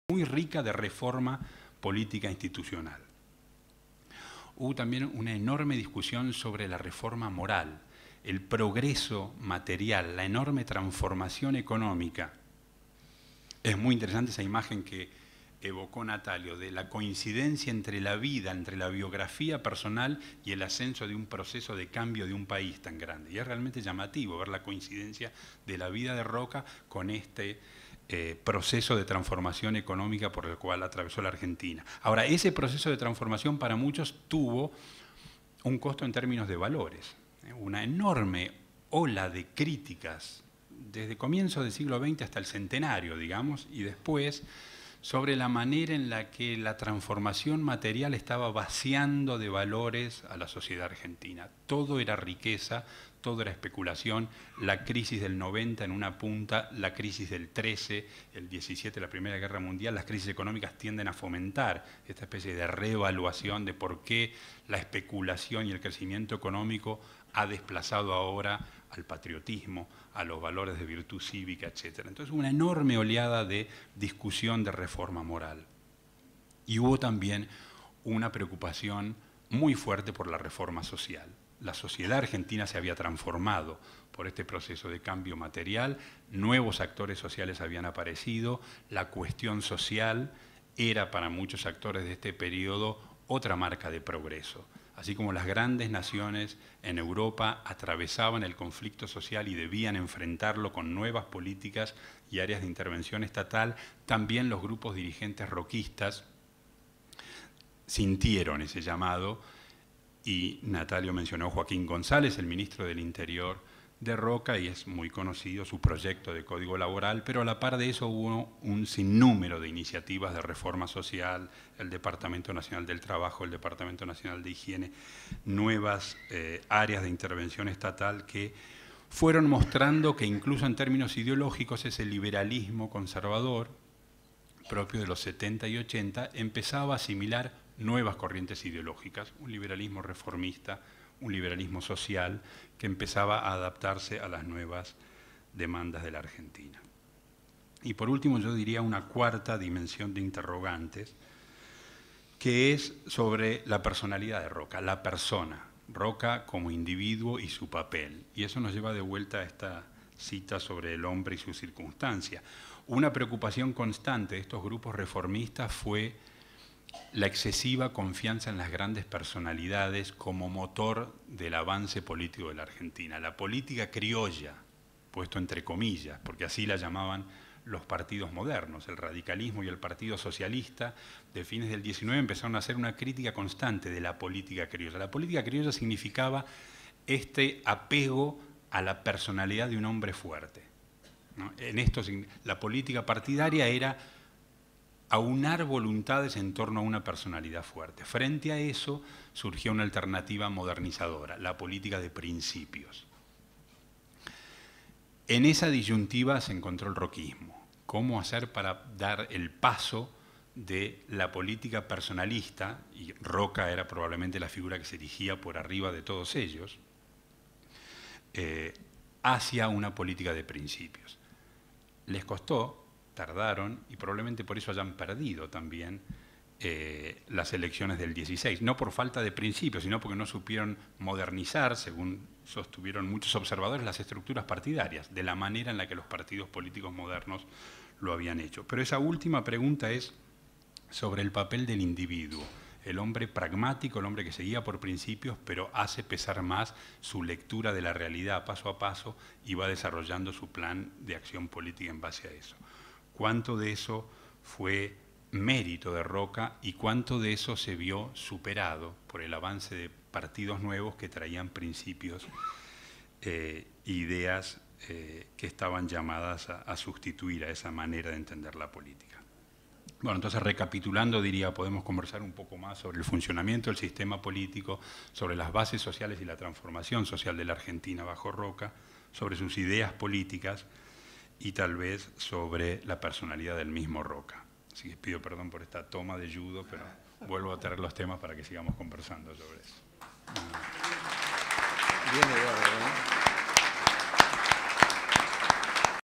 Roca, el régimen político y la reforma social (Jockey Club, 3/12/2014). Eduardo Zimmerman (Parte II)
"Roca, el régimen político y la reforma social". Disertantes: Natalio Botana y Eduardo Zimmerman. Miércoles 3 de diciembre de 2014, Jockey Club (Cerrito 1478, CABA), 19:30 hs. Disertación de Eduardo Zimmerman (Parte II).